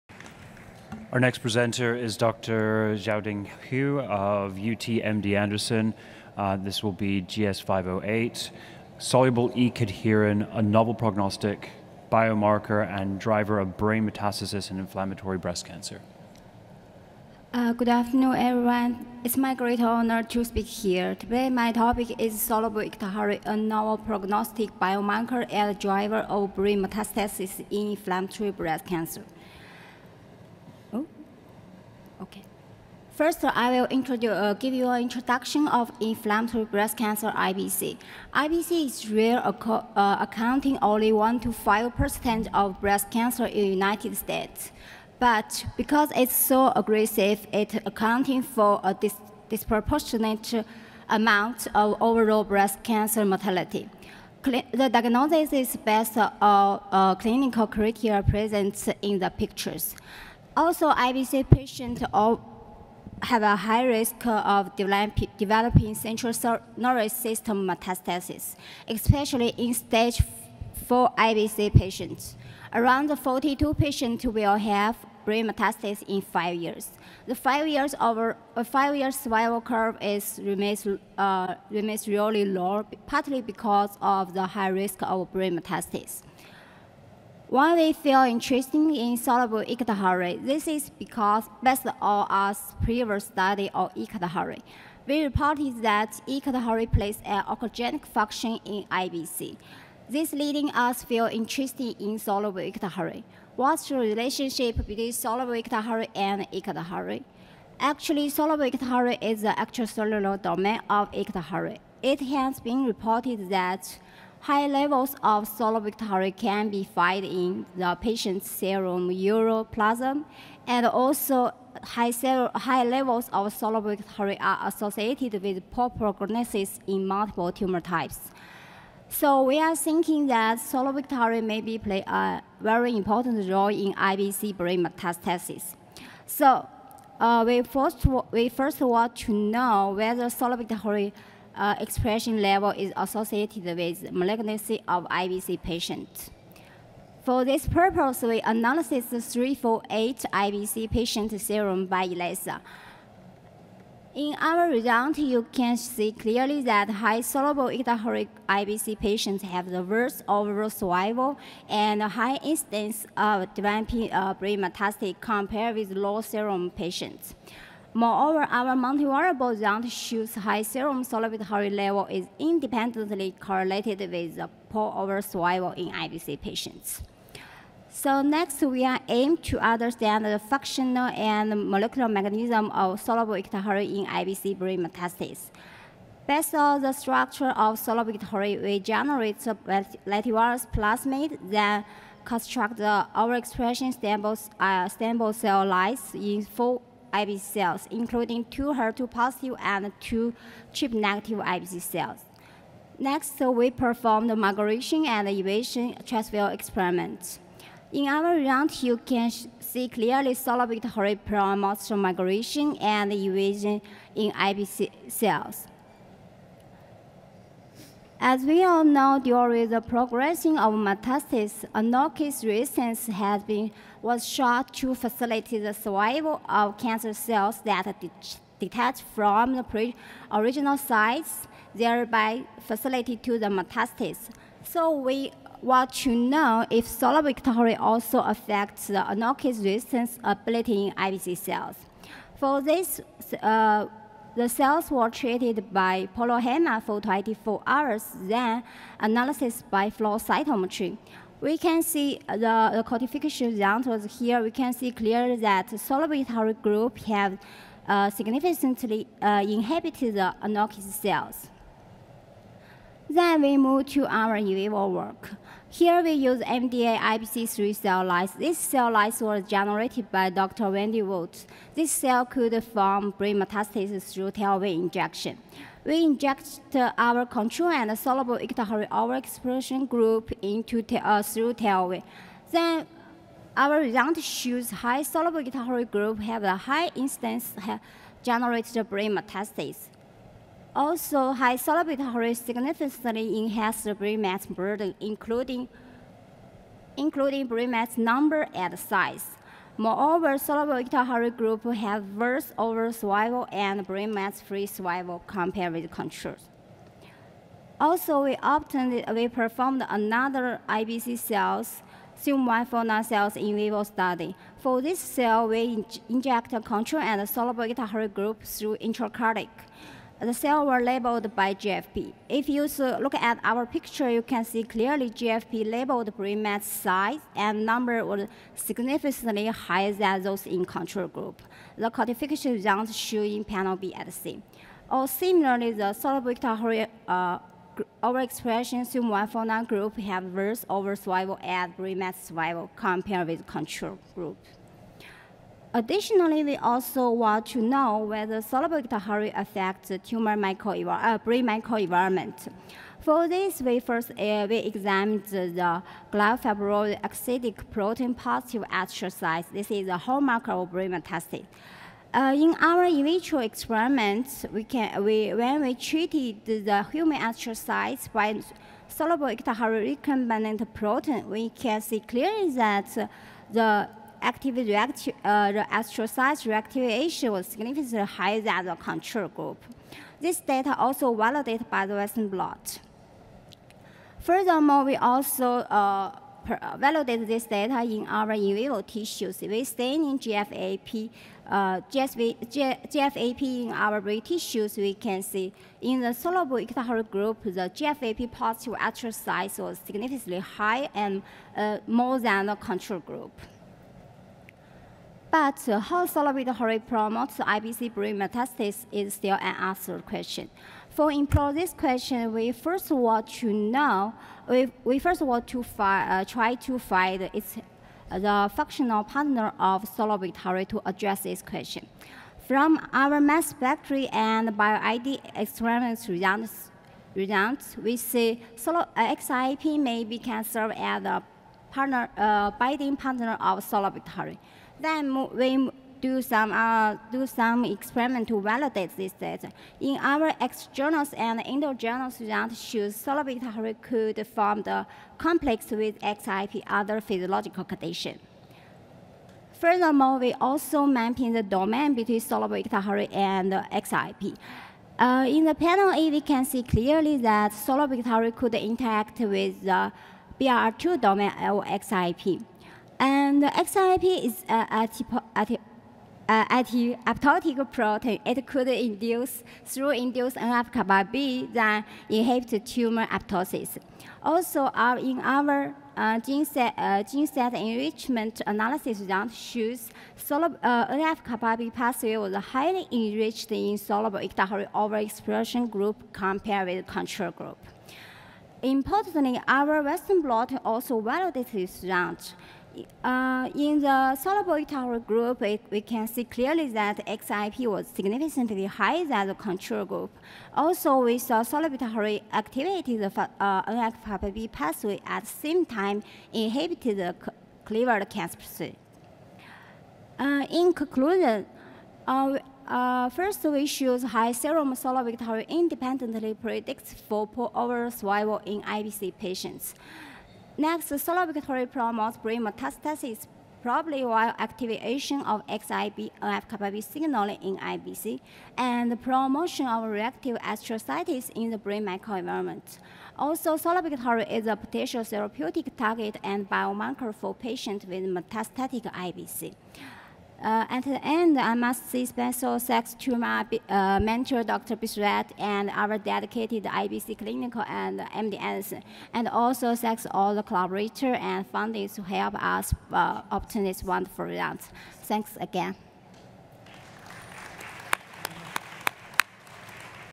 Dr. Xiaoding Hu | San Antonio Breast Cancer Symposium 2022
Dr. Xiaoding Hu speaking at the San Antonio Breast Cancer Symposium on Friday, December 9th 2022